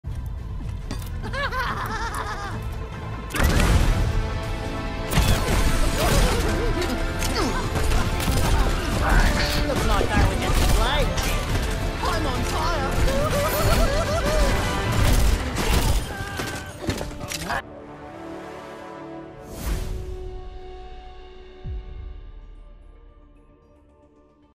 Fire at EVERYONE!!!
I should definitely work on my timing with my grenades, but those pesky snipers deserved the empty-the-clip routine, even if I can't aim!